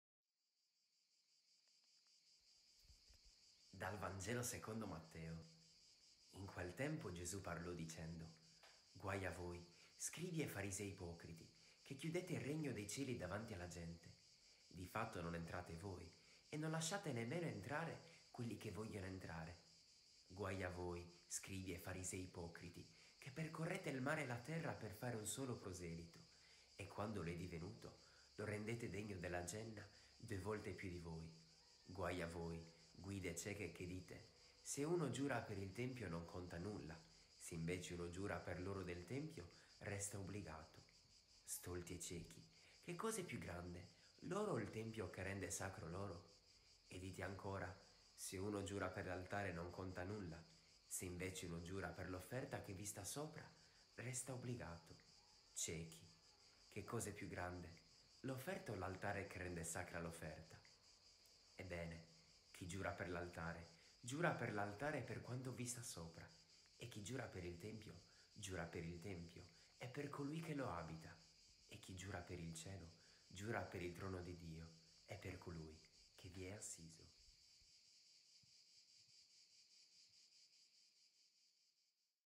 Vangelo del giorno - 26 Agosto - Mt 23,13-22
In quel tempo, Gesù parlò dicendo:
«Guai a voi, scribi e farisei ipocriti, che chiudete il regno dei cieli davanti alla gente; di fatto non entrate voi, e non lasciate entrare nemmeno quelli che vogliono entrare.
Guai a voi, scribi e farisei ipocriti, che percorrete il mare e la terra per fare un solo prosèlito e, quando lo è divenuto, lo rendete degno della Geènna due volte più di voi.
Guai a voi, guide cieche, che dite: “Se uno giura per il tempio, non conta nulla; se invece uno giura per l’oro del tempio, resta obbligato”. Stolti e ciechi! Che cosa è più grande: l’oro o il tempio che rende sacro l’oro? E dite ancora: “Se uno giura per l’altare, non conta nulla; se invece uno giura per l’offerta che vi sta sopra, resta obbligato”. Ciechi! Che cosa è più grande: l’offerta o l’altare che rende sacra l’offerta? Ebbene, chi giura per l’altare, giura per l’altare e per quanto vi sta sopra; e chi giura per il tempio, giura per il tempio e per Colui che lo abita. E chi giura per il cielo, giura per il trono di Dio e per Colui che vi è assiso».